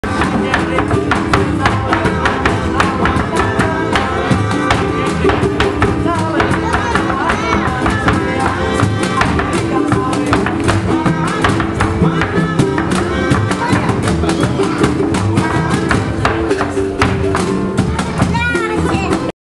Fandango De Primavera -- Patio Muñoz
Fandango De Primavera on Pino Suarez in Xalapa, Mexico